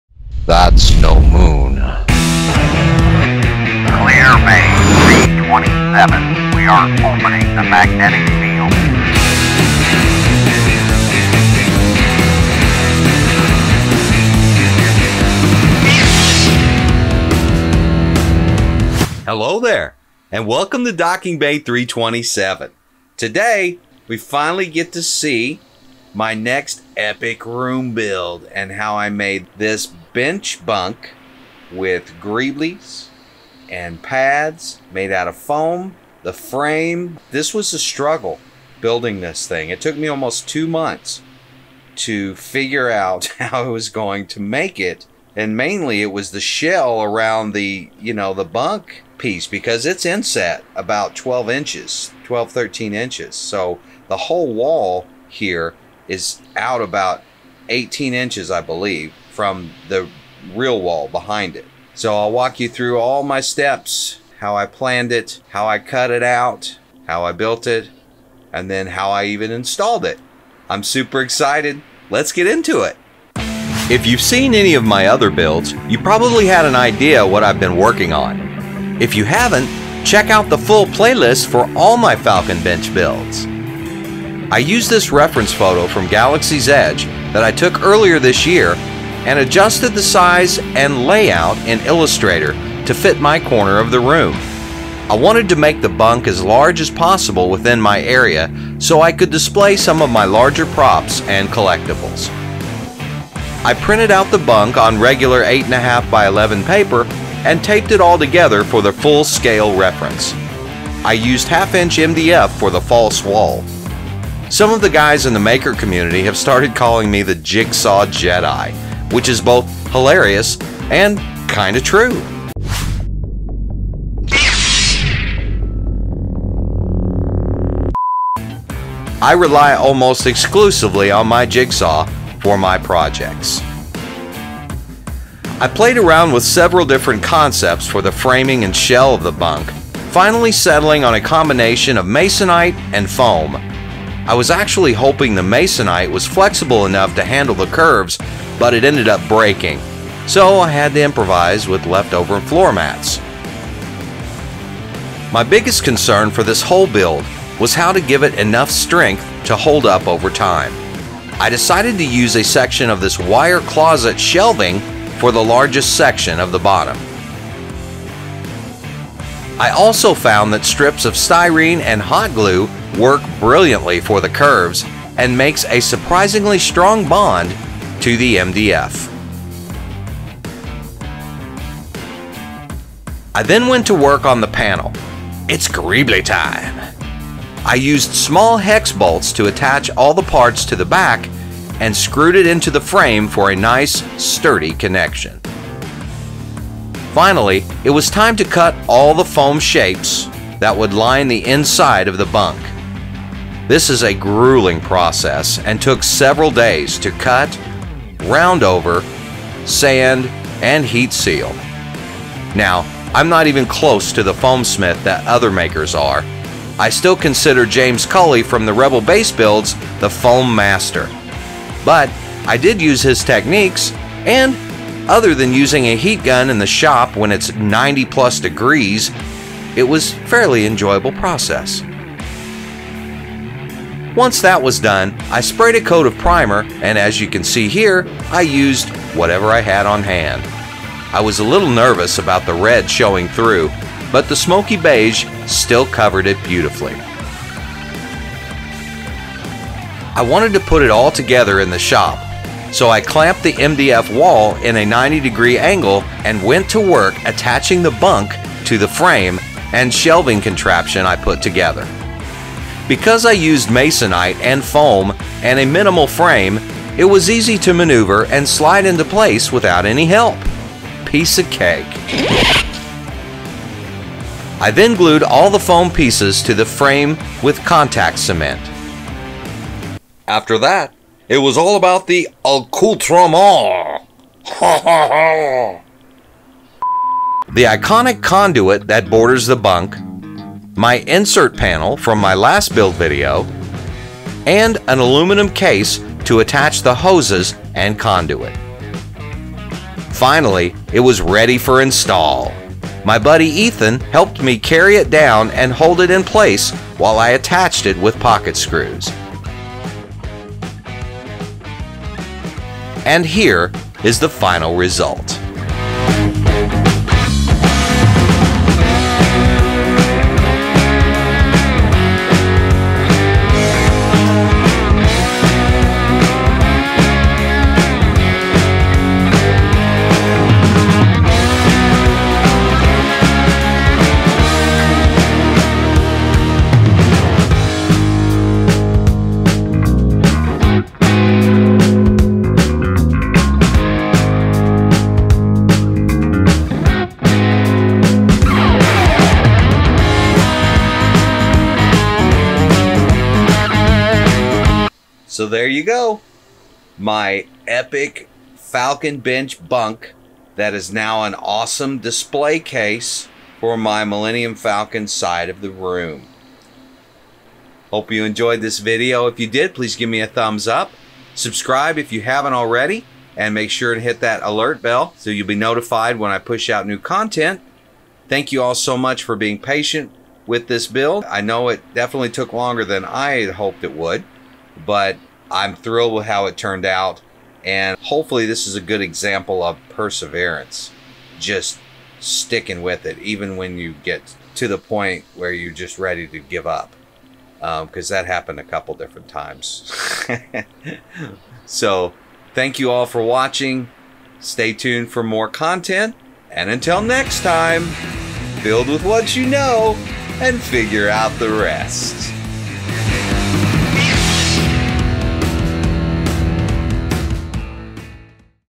Millennium Falcon Bunk - Build Video
Today we look at how I built a custom falcon bunk for my Star Wars room.

Like the videos? Would you like access to exclusive content, behind the scenes footage, digital downloads and more?